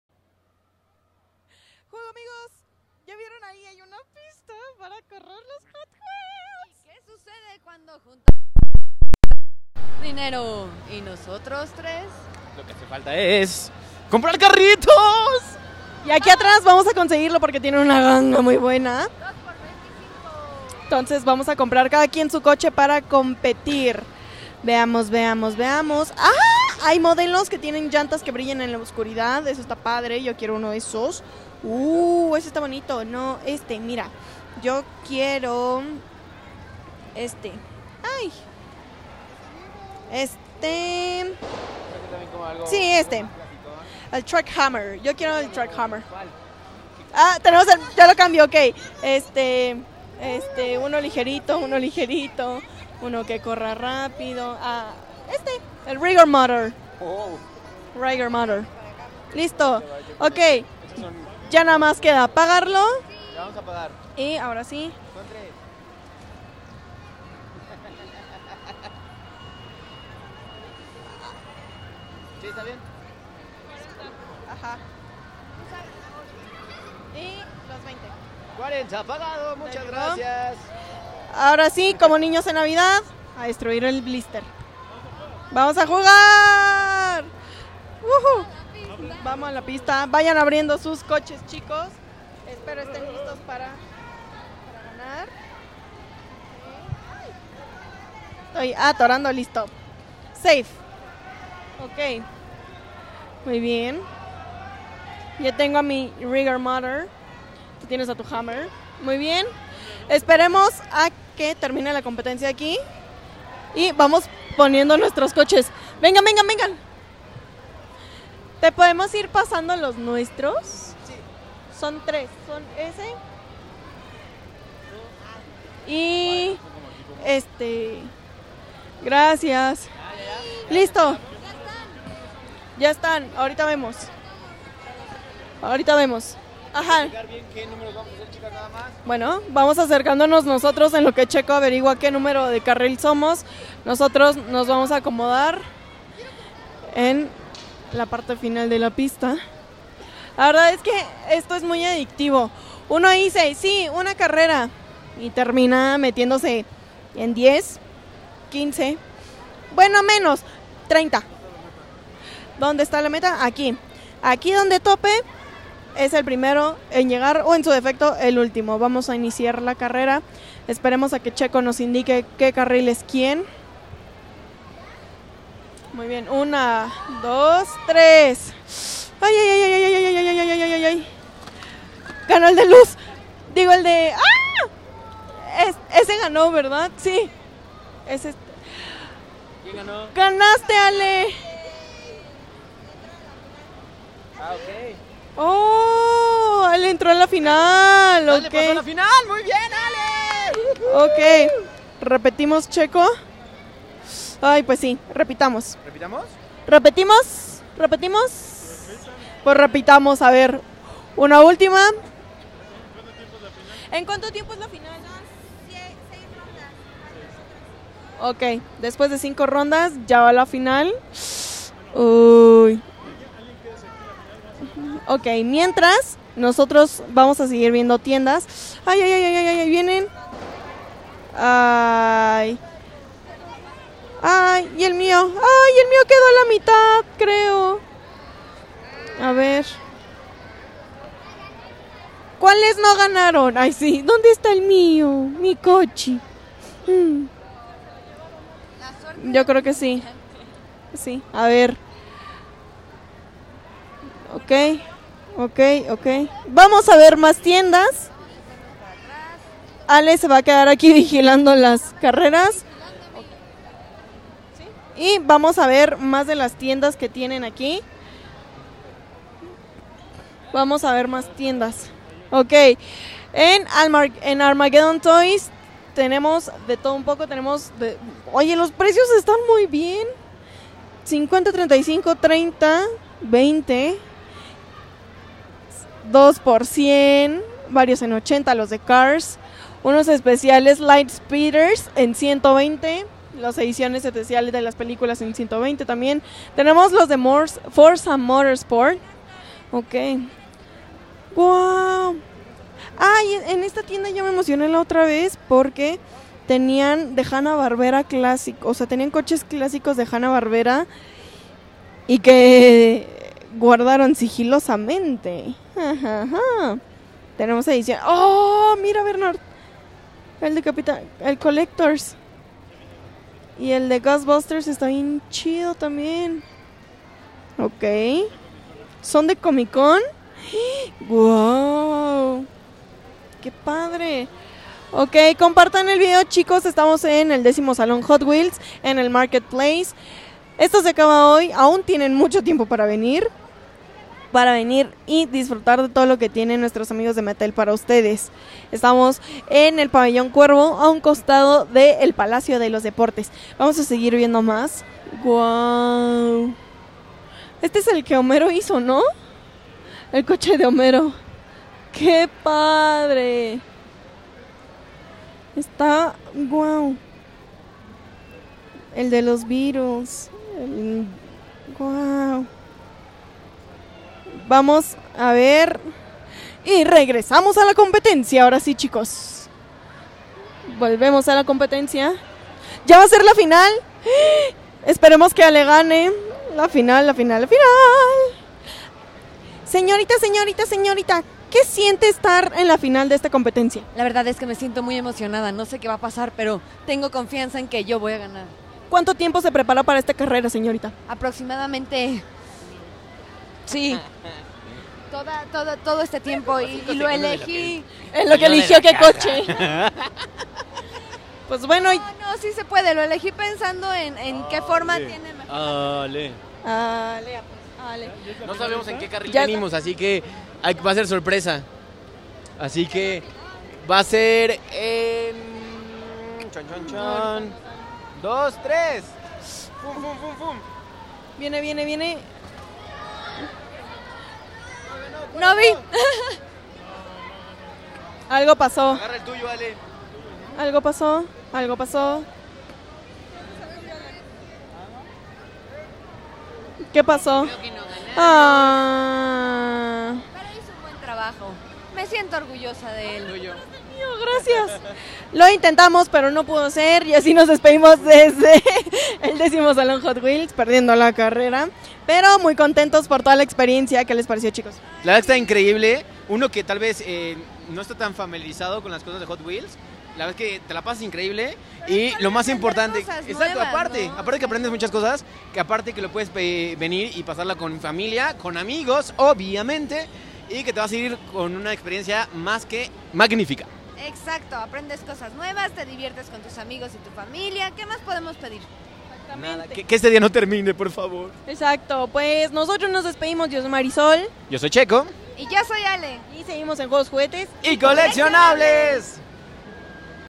10 Salon Hot Wheels Experiencia 3 final carreritas ★ juegos juguetes y coleccionables ★
y porque no jugar carreritas con la pista hot wheels, quien sabe igual ganamos  10 Salon Hot Wheels Experiencia 3 final